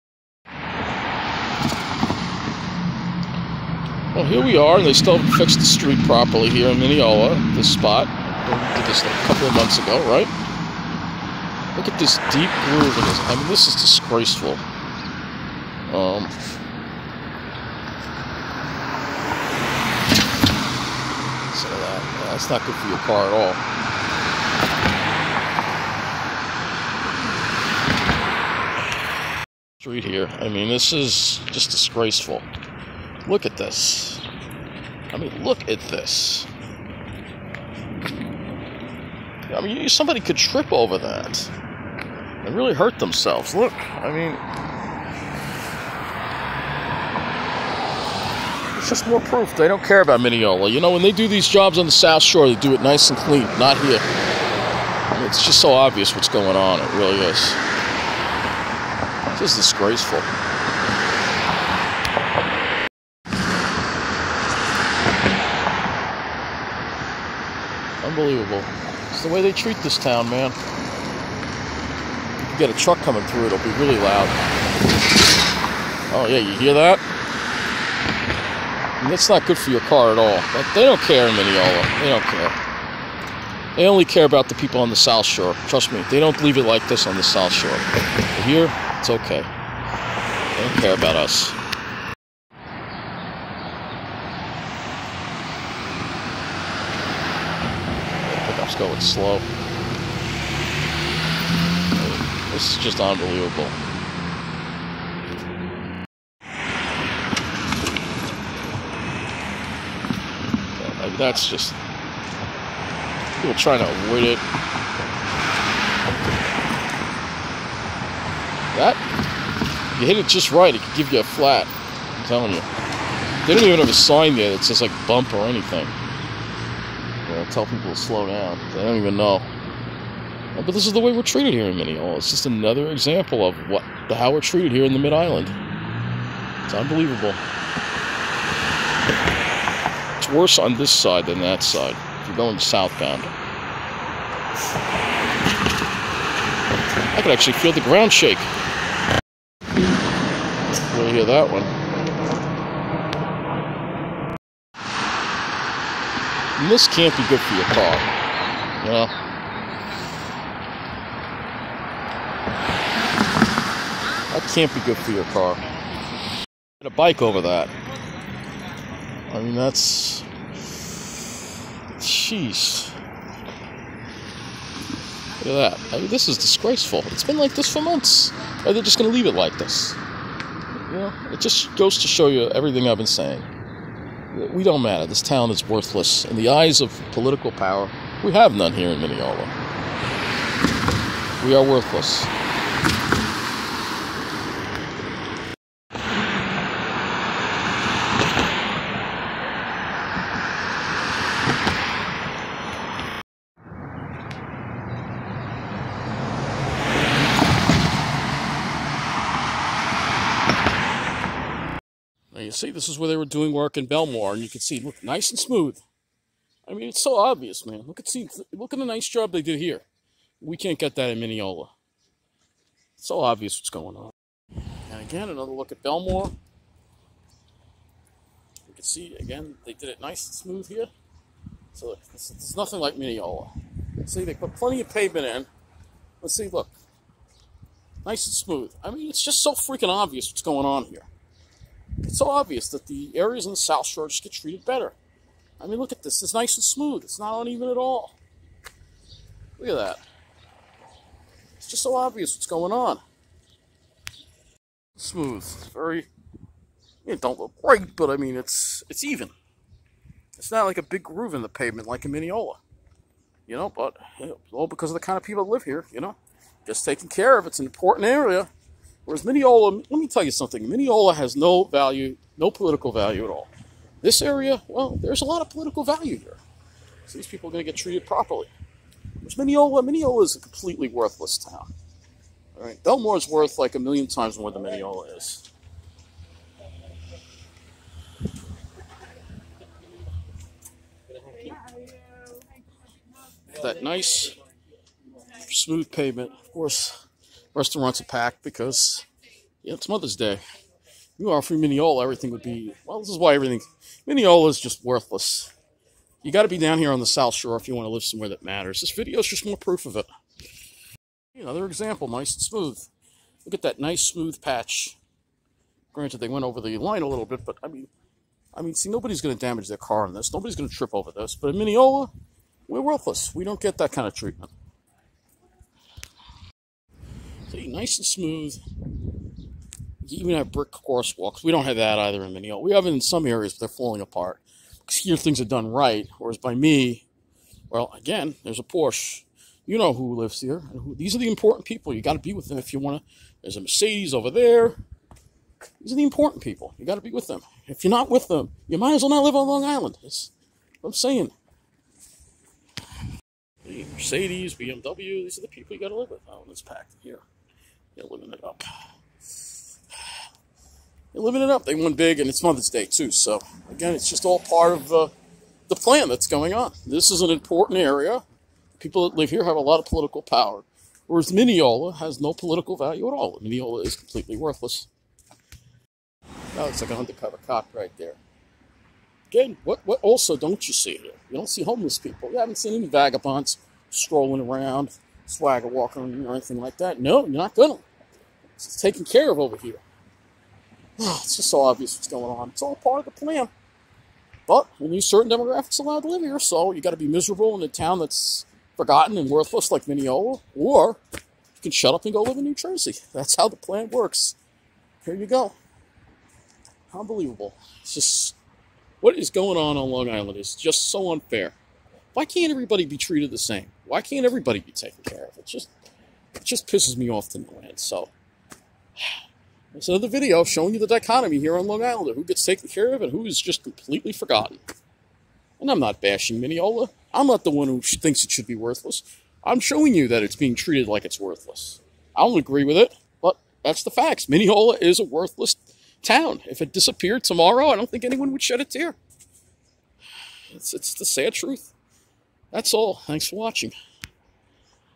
More Discrimination & Disparity: Gas Main Work Mess in Mineola vs Nicely Done in Bellmore
First upload is stuck at poor quality 360p.
National Grid is replacing gas mains across Long Island, but the subcontractors doing the work do a crappy job patching the pavement in the middle of the Island in Mineola, while making it nice & smooth for $outh $hore communities like Bellmore. Can't be any more obvious what's doing on!